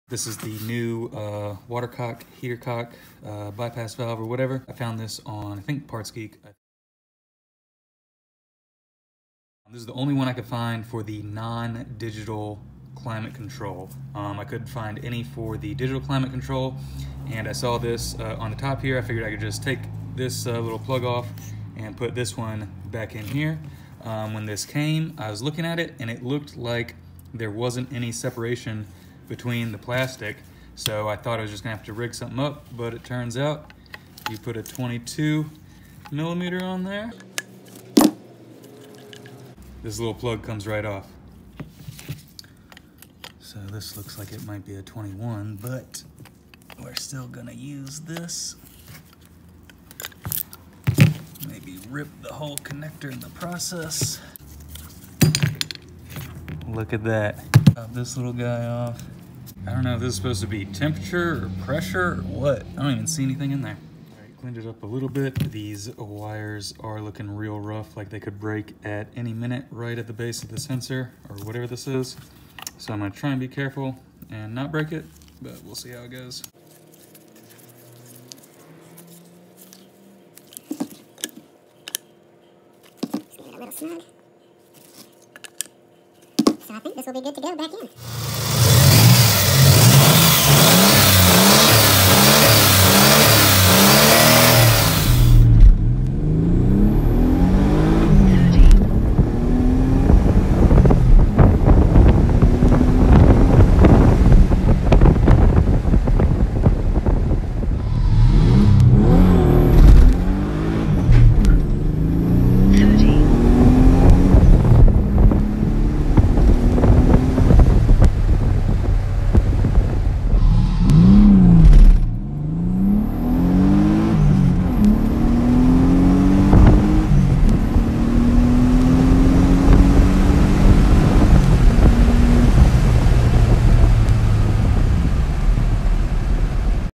Heater Valve for Digital Dash Z31 Ep 35
Finally time to install the heater valve / water cock / heater cock so that I at least have heat again. I could not find any heater valves for the digital dash, only for the analog. It looks like from the picture you can just unscrew the plastic plug on top and install the sensor for digi dash applications.

A few draggy runs too.

Link for heater valve (it came in OEM Nissan box and was cheaper than Nissan parts deal):